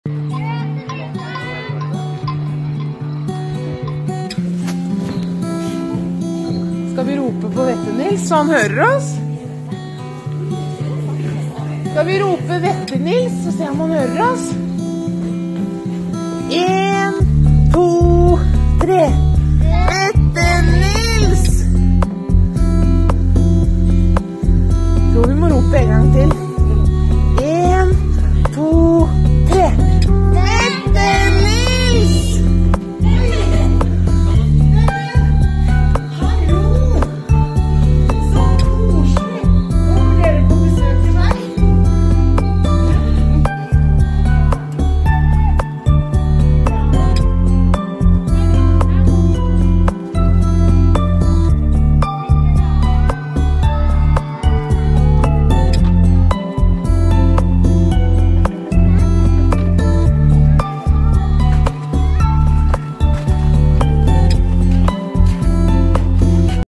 Julemarked 2018
Alltid julemarked første helgen i desember på Aktivitetsgården.  Christmas experience in Norway.